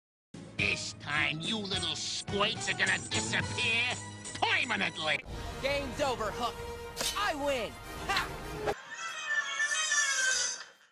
Peter Pan Throws his Dagger at The Beagle Boys
My 6th The Beagle Boys Video,Let's see how Peter Pan defeat the Beagle Boys.